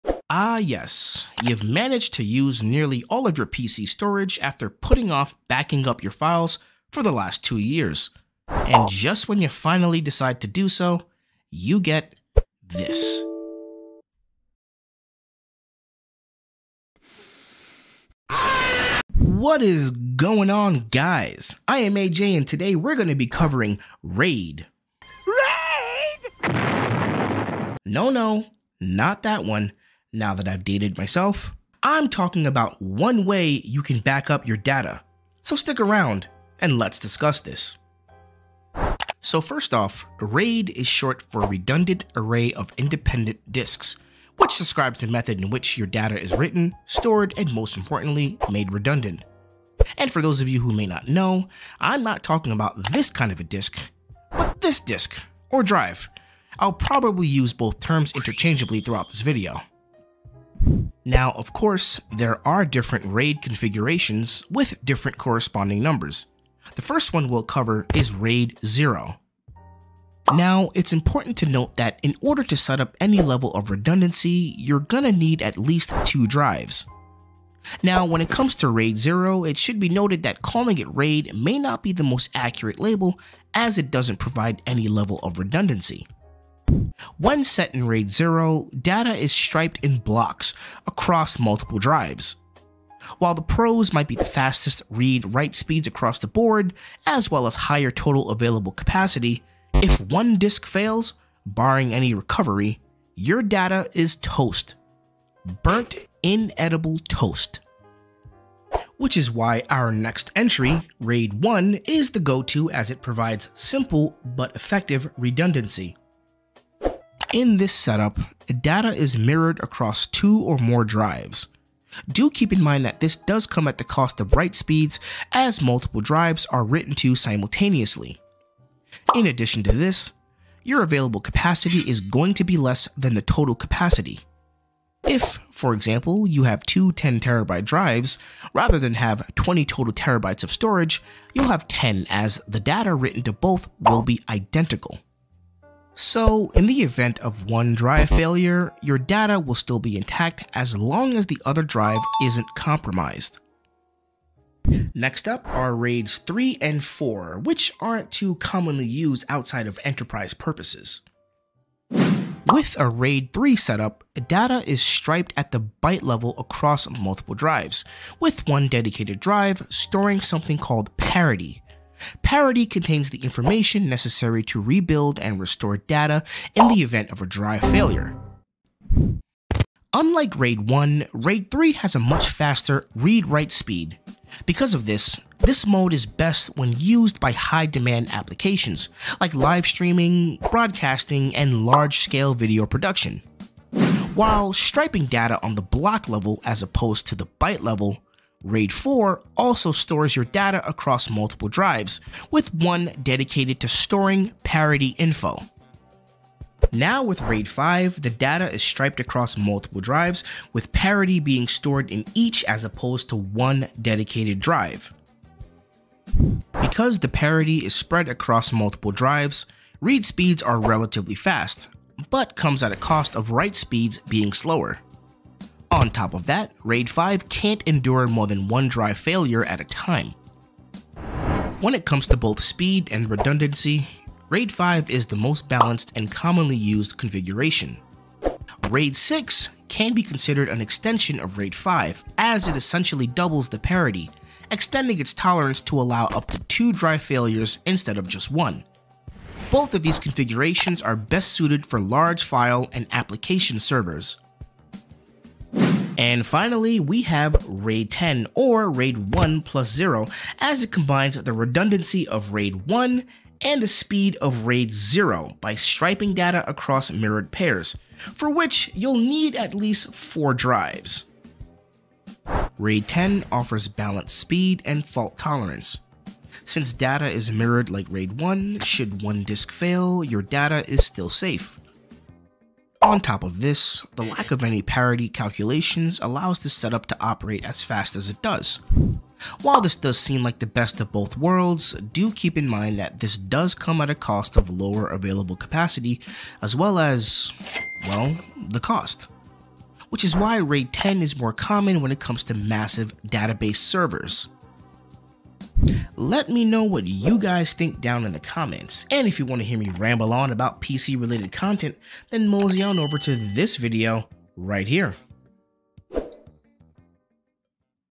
RAID Explained!
If you're new to RAID, then this video is for you! RAID (Redundant Array of Independent Disks) is a data storage technology that helps you protect your data from becoming unusable in the event of a disaster.

DMCA takedowns are illegal and will be contested.